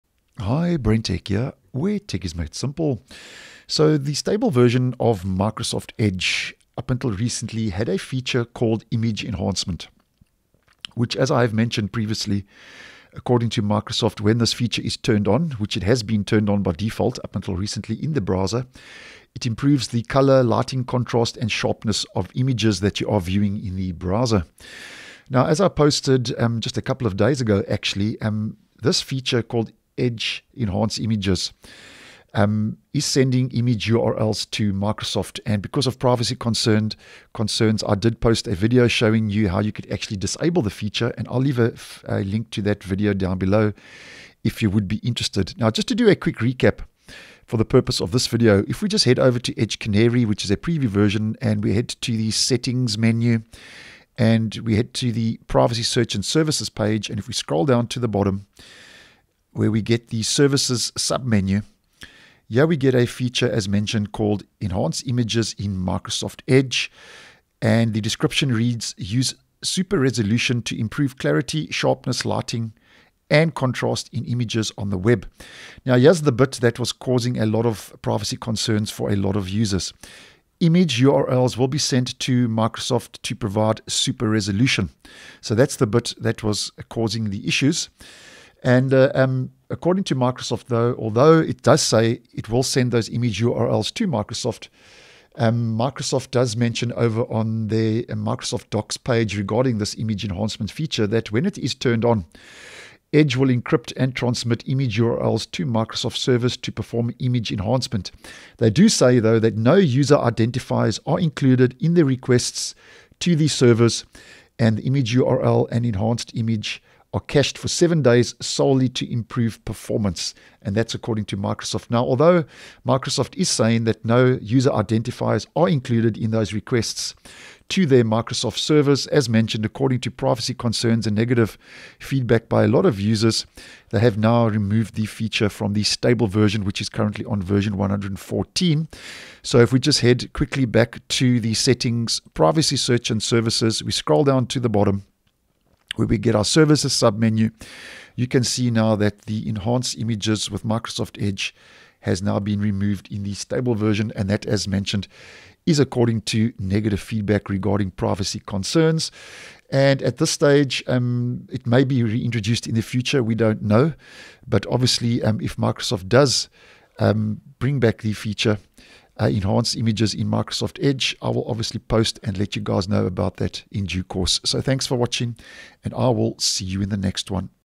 UPDATE Microsoft has removed Edge’s Enhance images feature, because of privacy issues
As previously posted, there is a setting to disable/enable Enhance images in Microsoft Edge, that sharpens color, lighting and contrast In Edge Canary, this feature had been updated to make it clear that "image URLs will be sent to Microsoft to provide super resolution", which for some users was a privacy concern. In the stable version of Edge 114, Microsoft has now removed the Image Enhancement feature, due to the above mentioned privacy concerns.